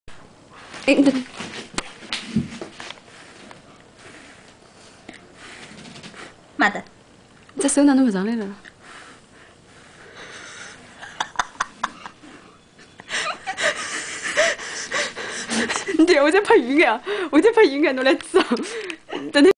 Torturing NONO
See how my puppy react to wind...